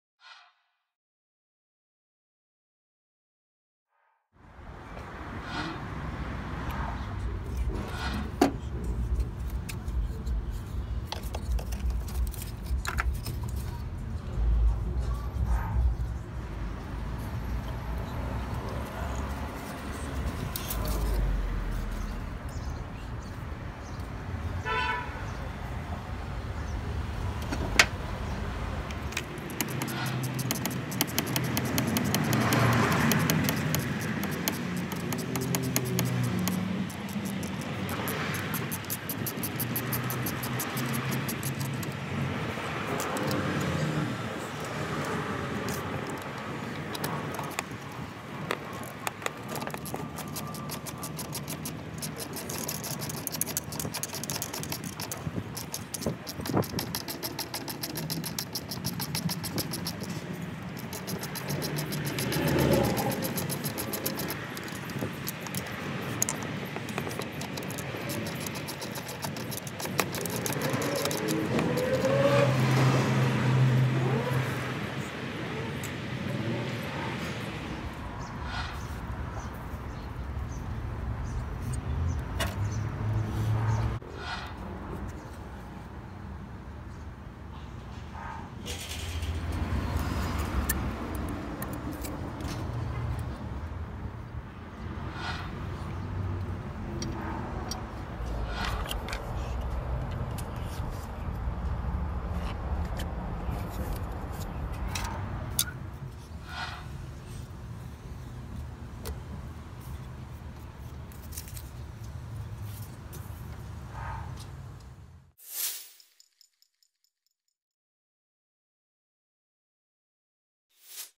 How to REMOVE the EGR Valve on a Ford Mustang (05-09 || V6)
Hey guys it’s Anthony here I just decided to make a video on how to remove the EGR valve, because I noticed theirs not that many videos in how too. My car is a 2005 Ford Mustang V6. Hope the video was good and useful, if it was please remember to drop a like down below.